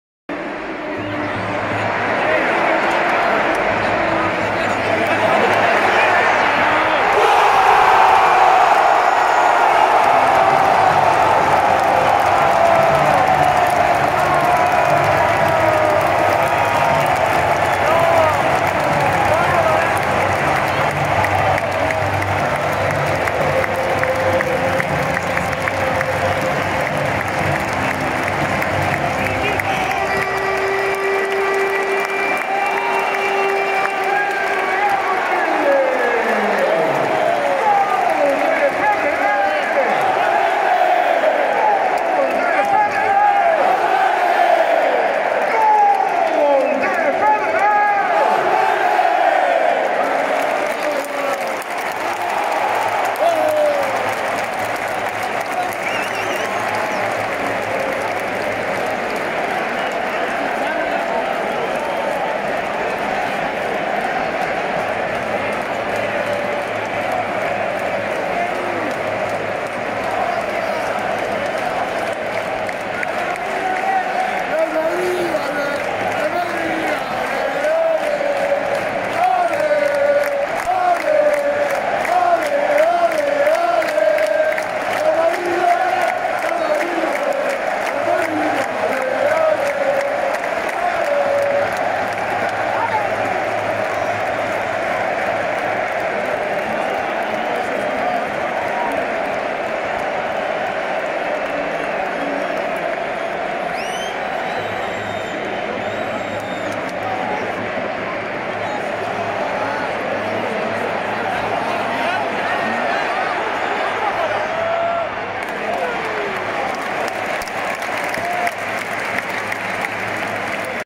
Real vs. Barca I 2-0 Valverde! Brilliant 20 meter shot I El Clásico Oct 22
Mendy passes the ball back to Valverde. He aims from 20 meters and converts with a precise shot into the bottom left corner of the goal.

Real Madrid  3-1  FC Barcelona

El Clásico No.185 I La Liga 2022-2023
Round 9 I 16 October 2022
Estadio Santiago Bernabéu, Madrid

1-0 Benzema 12'
2-0 Valverde 35'
2-1 Torres 83'
3-1 Rodrygo 90+1'

Mendy devuelve deliberadamente el balón a Valverde. Toma medidas desde 20 metros y convierte un tiro preciso en la esquina inferior izquierda.